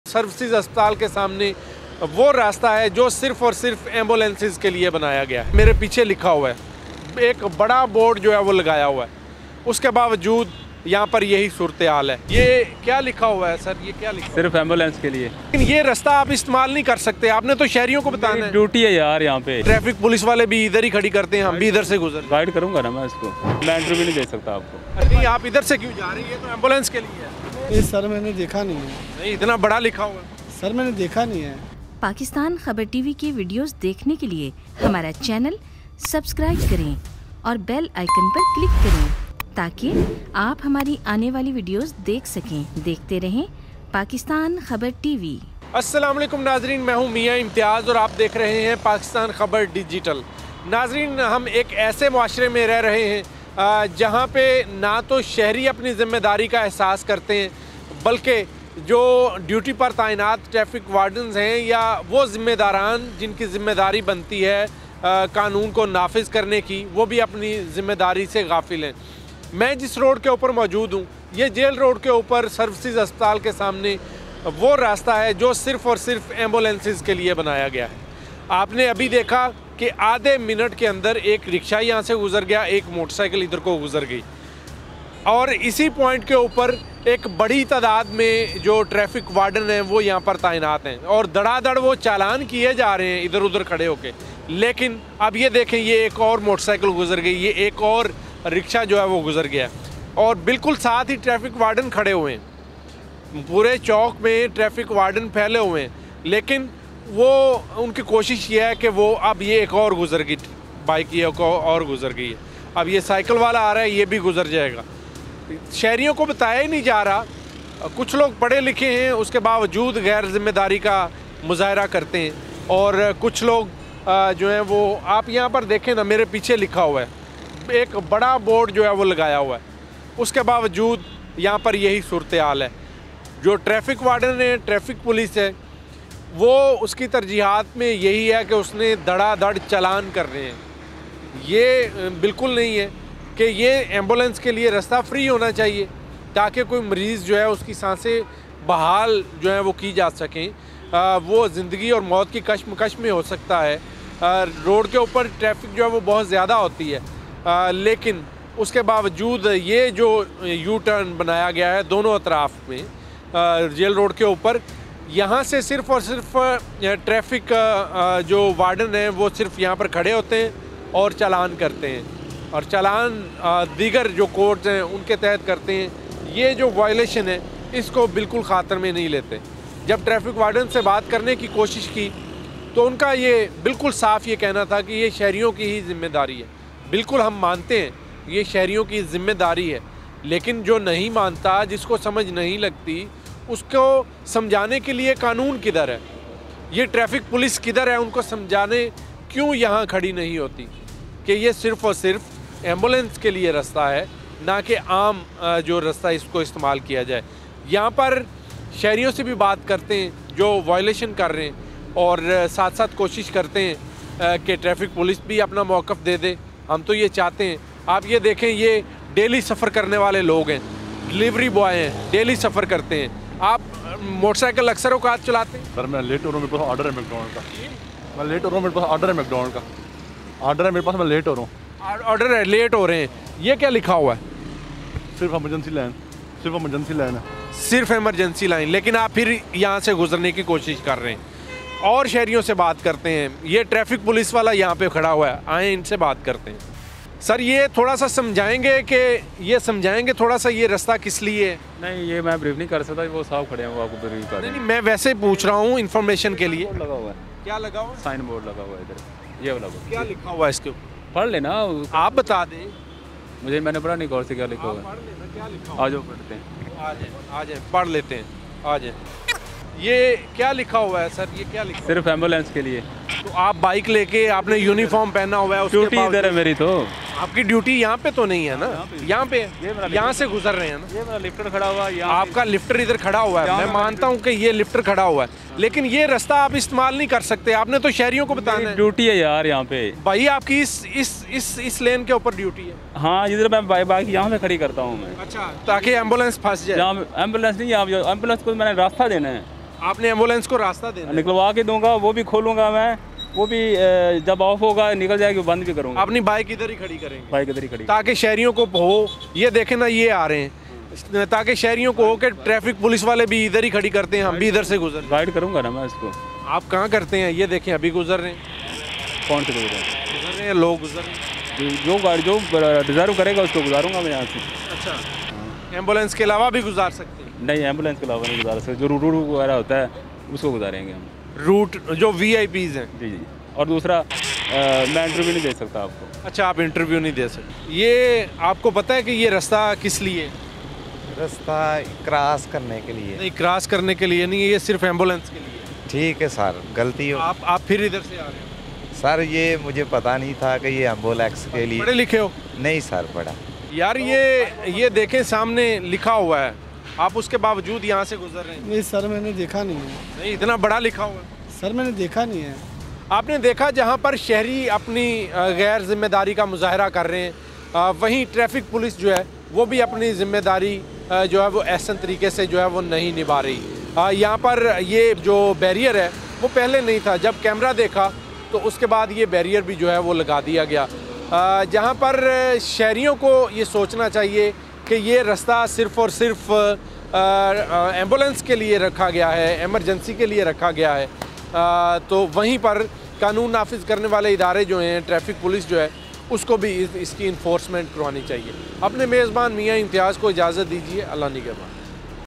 Ill-Informed Public and Weak Traffic Police at Intersections | Unveiling Chaos | Report Mian Imtiaz
Join us in exposing the alarming scenario where an ill-informed public and weak traffic police contribute to chaos at crucial intersections. Discover the repercussions of people misusing lanes designated for ambulances, leading to traffic nightmares.